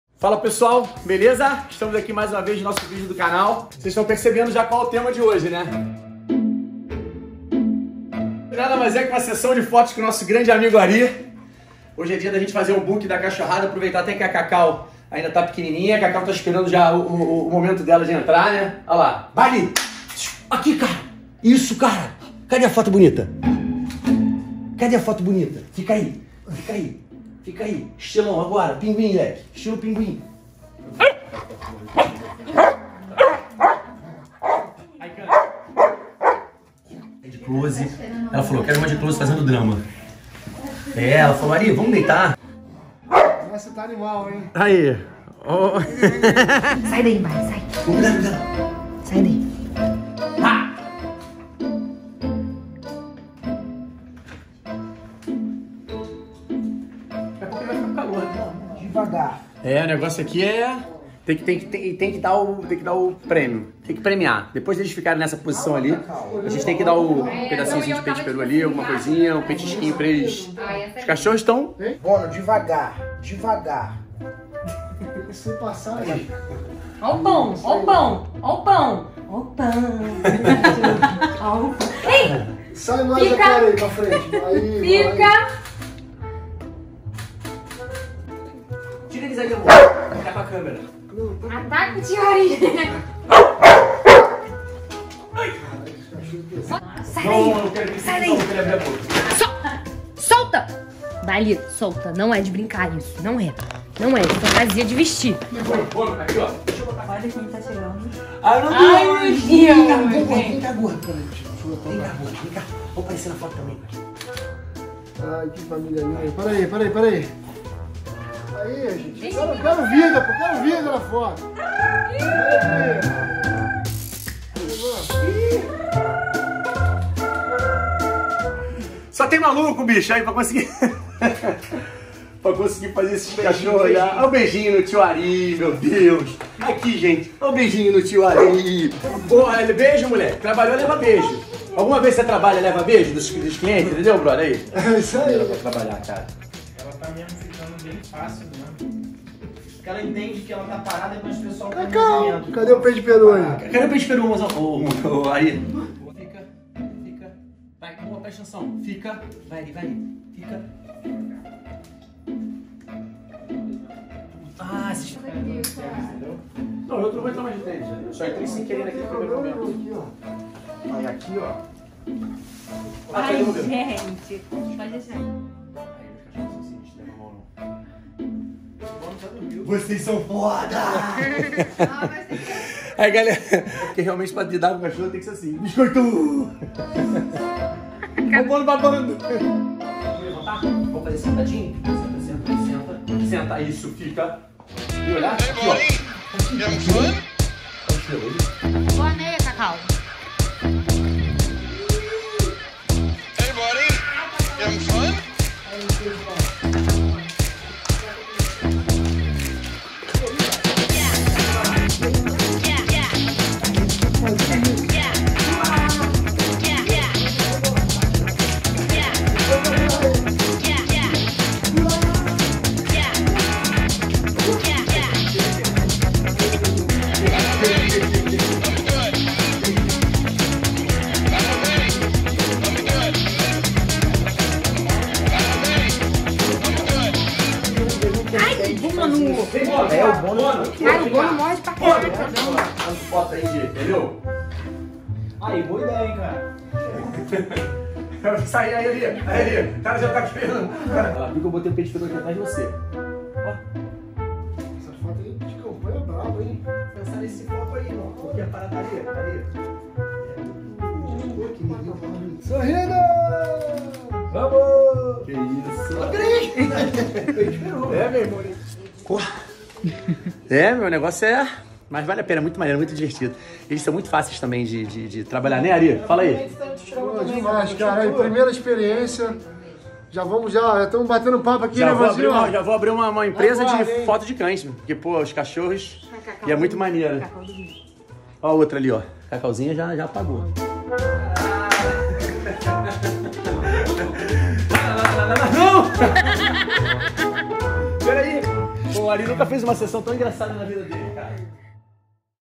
Fizemos um BOOK de fotos com os CACHORROS. Olha como foi! 🐶 📸 | Bono Surf Dog
Levamos a criançada para fazer um book super legal, com figurinos e tudo. Vem ver a reação deles na hora de tirar as fotos! 🐶📸

🏆Penta campeão mundial de Surf Dog ⠀⠀⠀⠀⠀
🇧🇷Guinness World Record holder